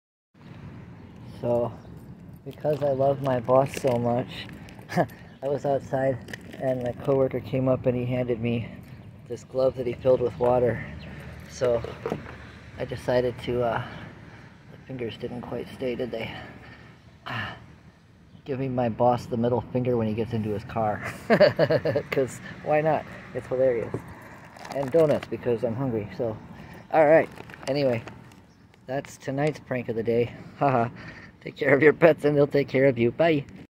I Pranked My Boss- Haha! 😂🤣
So my co-worker walked up to me and handed me this glove full of water. I decided to put it on my boss's windshield and said it so that it's giving him the finger. Lol! 😂🤣

Okay, so my boss being my boss, there is an update video coming as soon as customers settle down again.